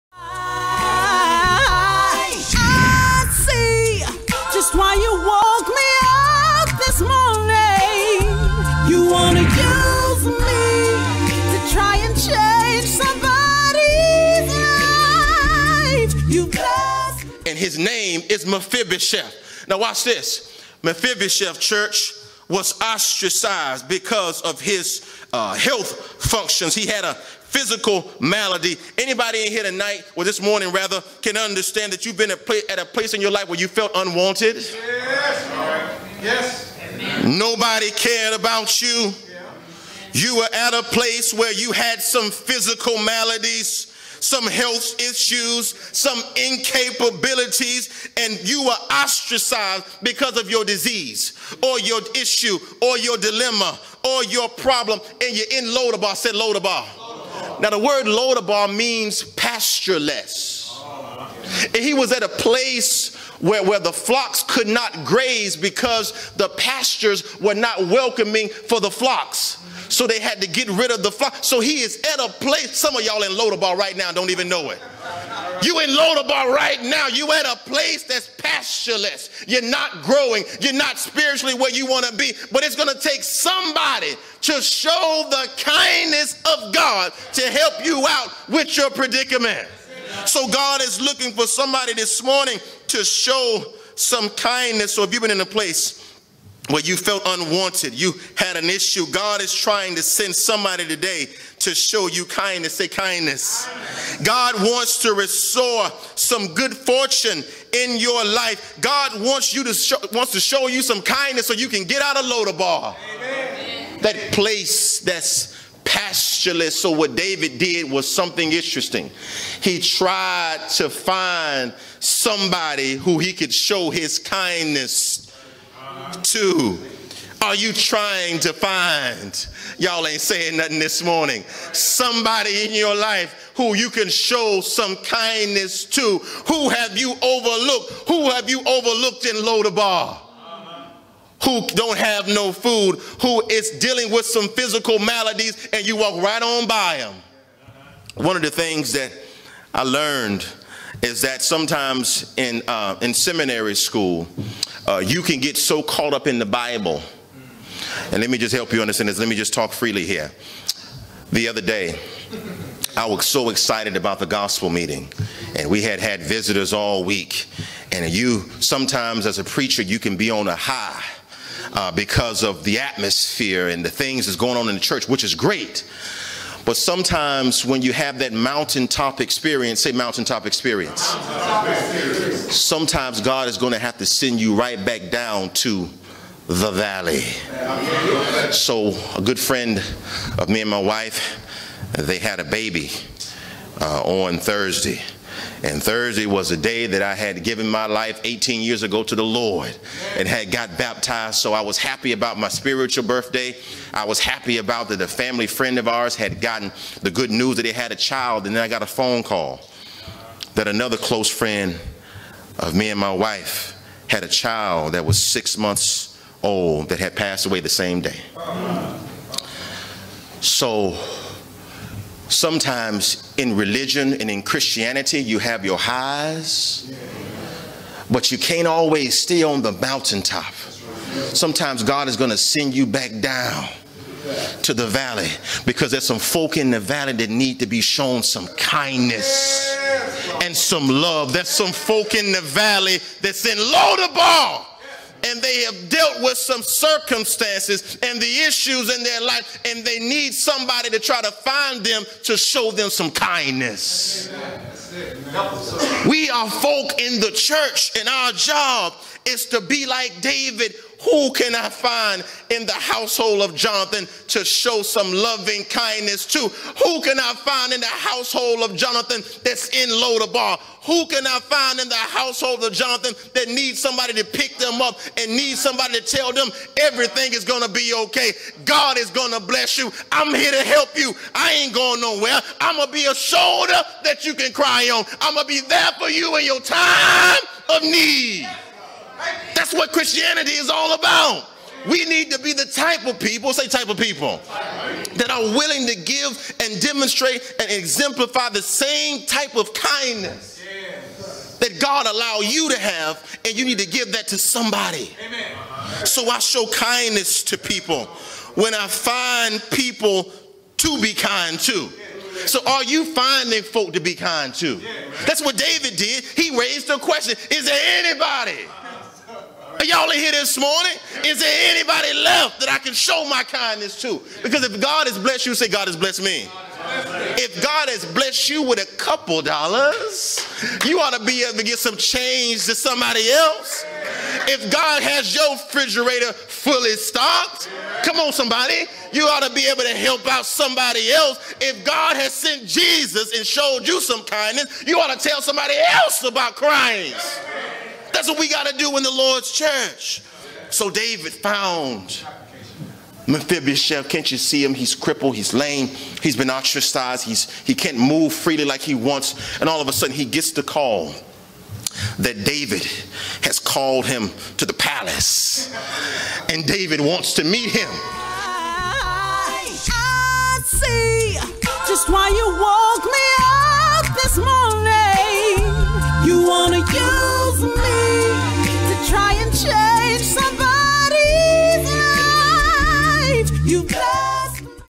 MORNING DEVOTION: Bryan C. Jones - " A SEAT AT THE KING'S TABLE"
This video is about GV COC Morning Devotion